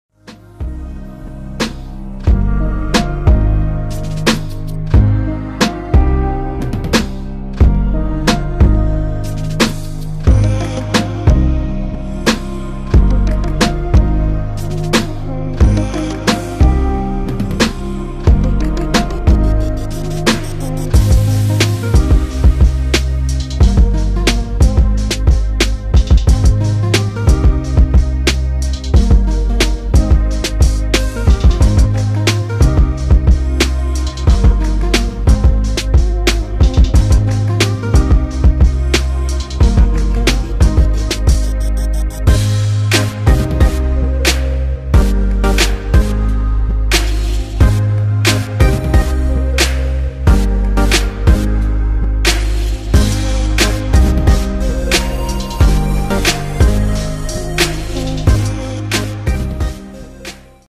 Latest gold mangalsutra designs with weight & price/gold mangalsutra mini ganthan design 2022
In this video we will see Latest gold mangalsutra designs with weight & price, gold mangalsutra mini ganthan design 2022